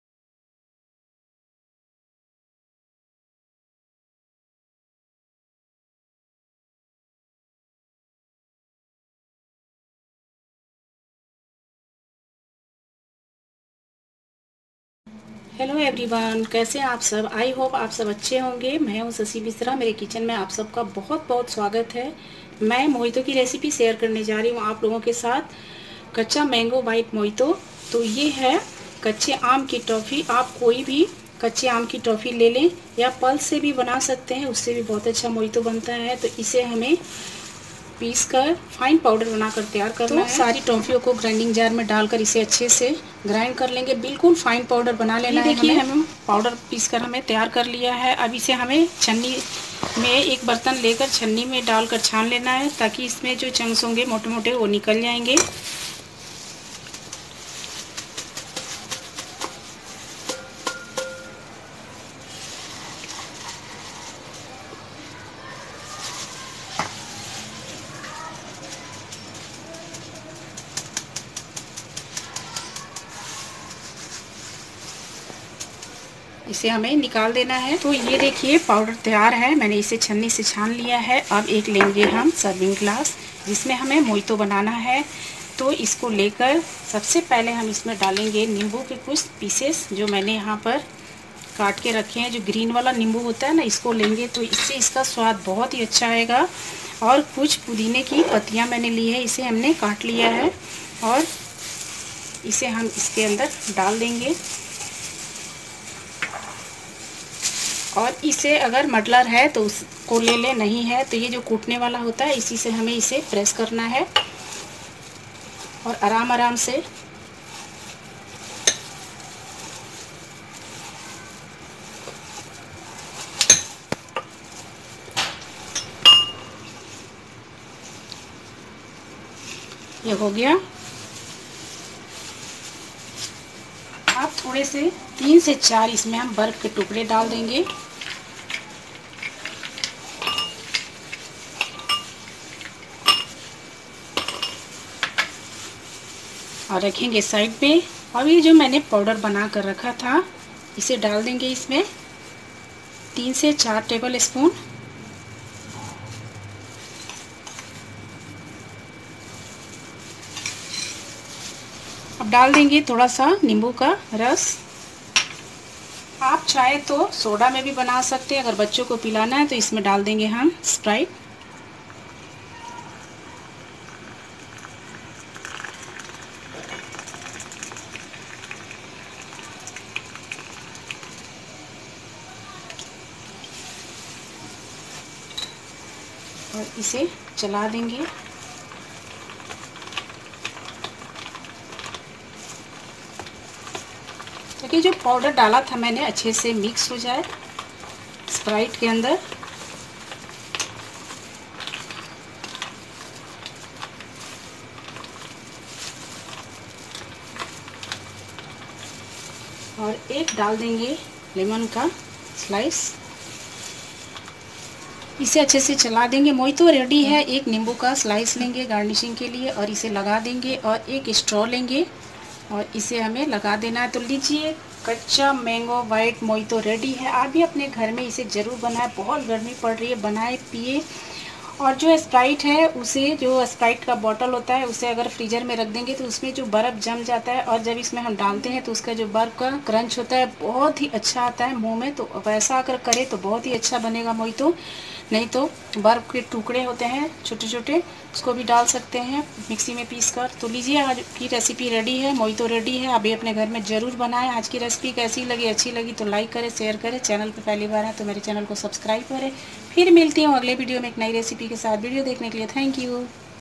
कच्चा मैंगो बाईट मोहितो || kaccha mango bite mojito || mojito recipes || unique recipes
Ingredients :

▪︎250 ml sprite or soda
▪7 to 8 mint leaves
▪︎20 to 25 kaccha mango bite toffee powdered
▪︎1 tbsp lemon juice
▪︎4 lemon small pieces
▪︎2 lemon slices
▪︎4 to 5 ice cubes